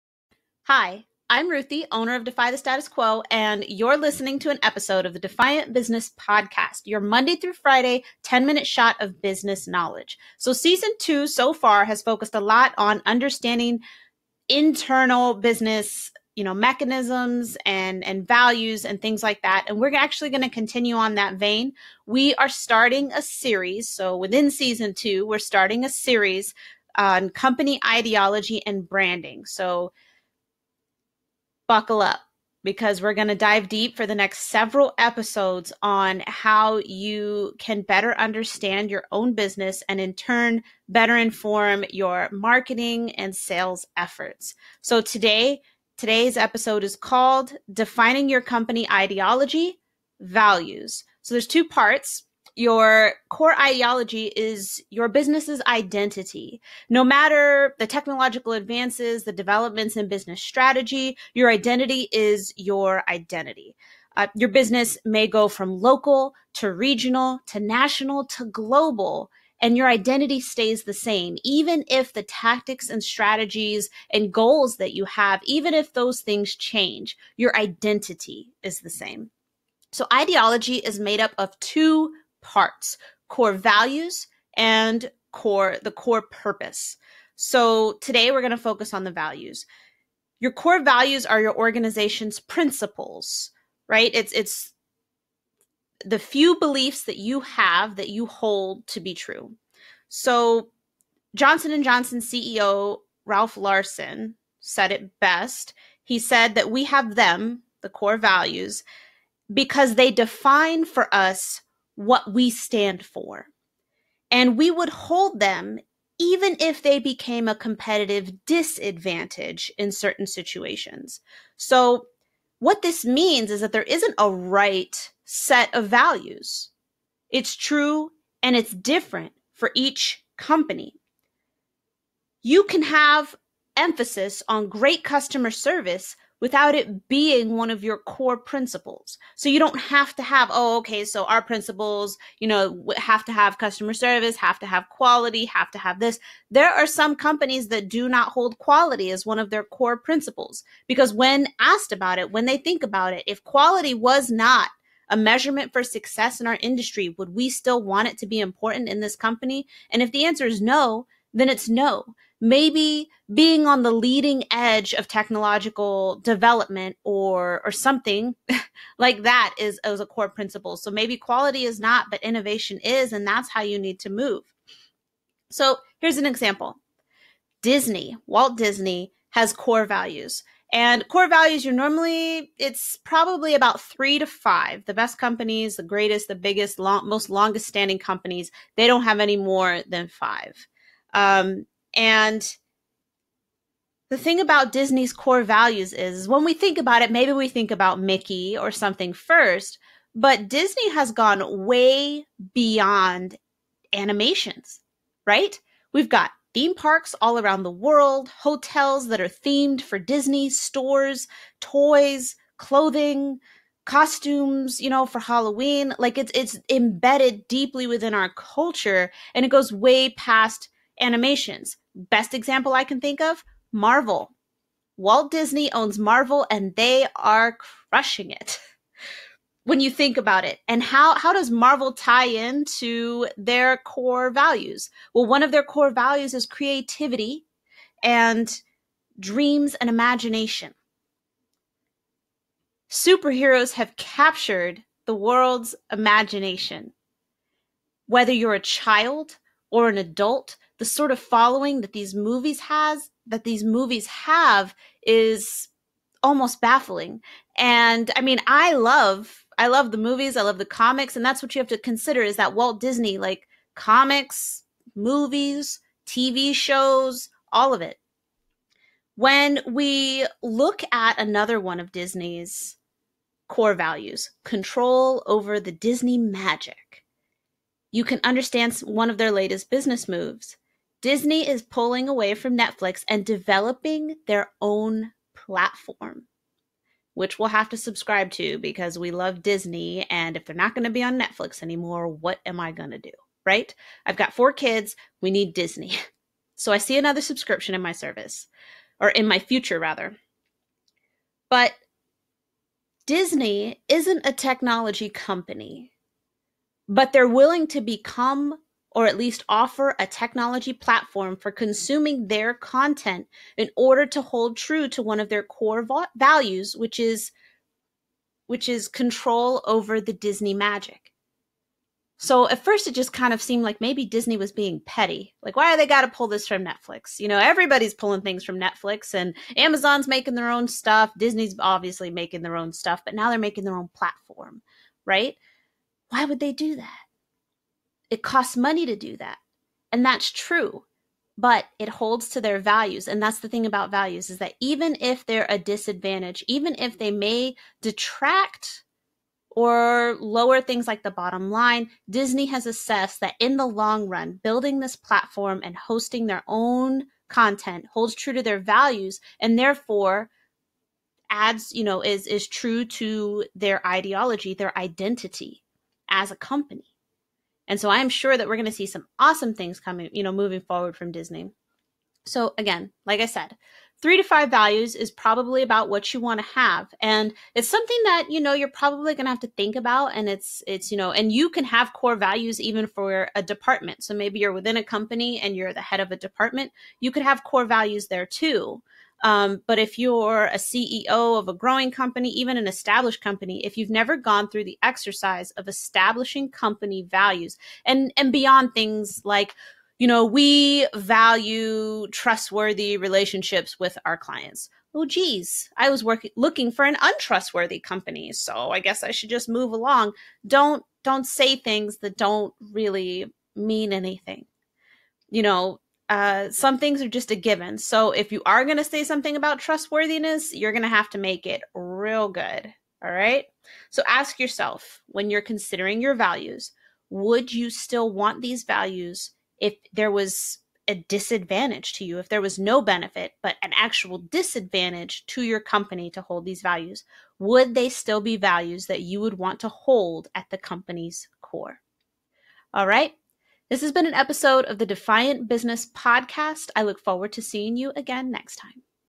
Ep. 9: Defining Your Company Ideology: Values
Your company's core ideology is your business' identity. Strip away the business goals, strategies, methodologies, and more, and this is what's left. Your ideology is made up of your core values and purpose. Today, we're going over values and how you can develop them for your company.